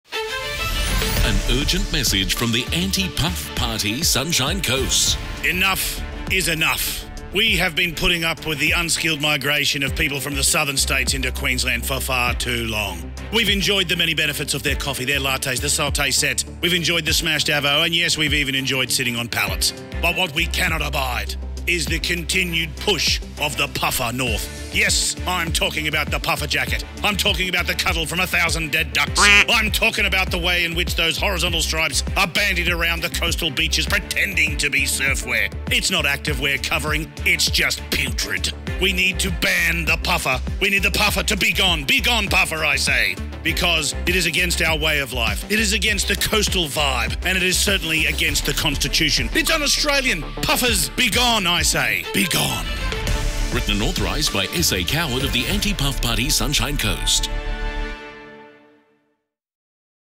The Anti-Puff Party 2023
Who wants to join Sam's Anti-Puff Party and make the Sunshine Coast a Puffer Jacket Free Zone?